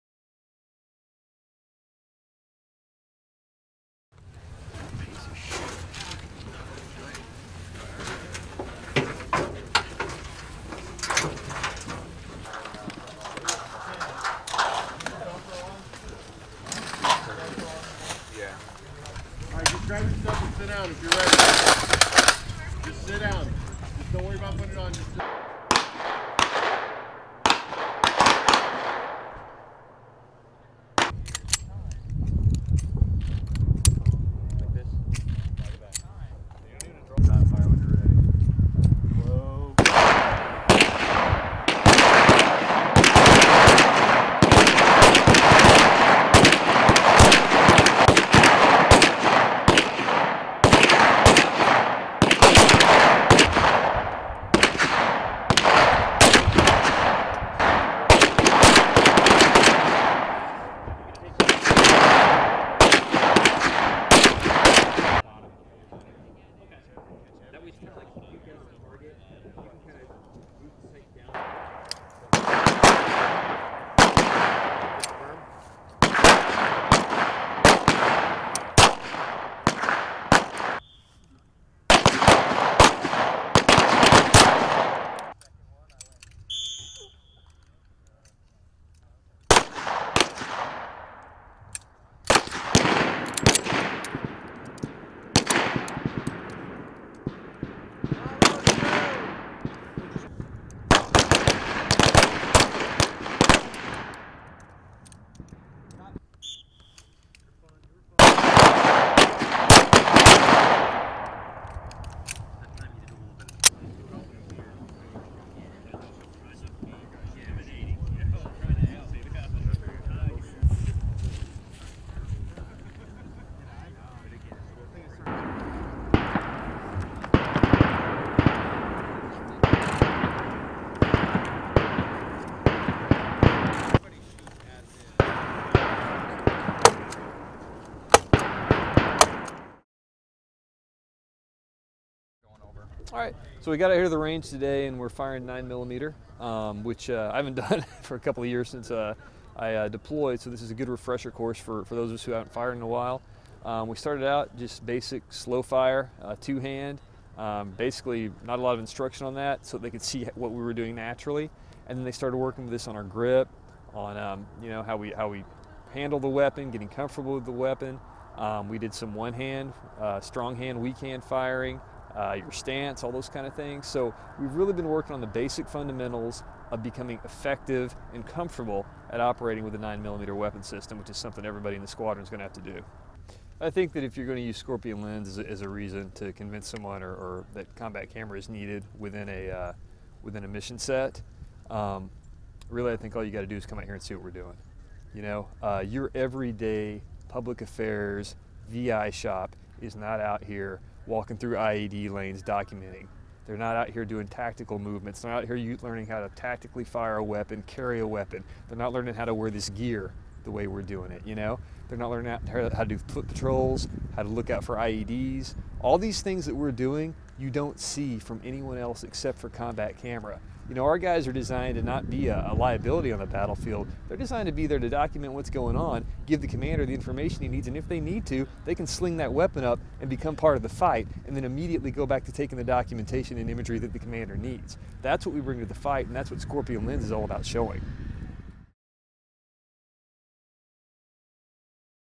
Scorpion Lens 2016 1
Credit: Airman 1st Class Jacob Albers | Date Taken: 03/03/2016
Airmen of the 1st Combat Camera Squadron, 3d Combat Camera Squadron and Operating Location Hill conduct weapons familiarization training with the M-9 Berretta at firing range 6 on Fort Jackson, S.C., March 3rd, 2016. Footage also includes an interview with Captain Zachary L. Anderson.Exercise Scorpion Lens is an annual Ability to Survive and Operate training evolution mandated by Air Force 3NOXX Job Qualification Standards (3NOXX AFJQS). Individuals are instructed using a, "crawl, walk run," format of training. The exercise is twofold containing the Scorpion Lens portion, dedicated to Advanced Weapons and Tactical Training (AWTT) and the Flash Bang portion dedicated to providing photography and videography documentation standards in combat situations. The purpose of the training is to providing photography and videography documentation standards in combat situations. The purpose of the training is to provide refresher training to combat camera personnel of all rank and skill levels in basic tactics, techniques and procedures inherent to combat camera mission tasking.